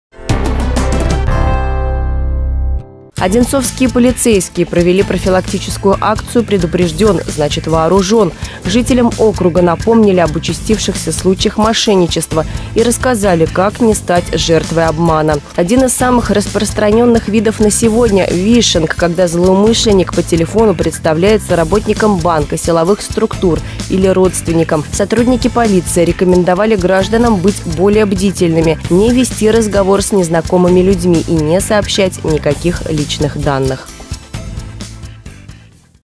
Предупрежден – значит вооружен!
Одинцовские полицейские провели профилактическую акцию «Предупрежден – значит вооружен!». Жителям округа напомнили об участившихся случаях мошенничества и рассказали, как не стать жертвой обмана.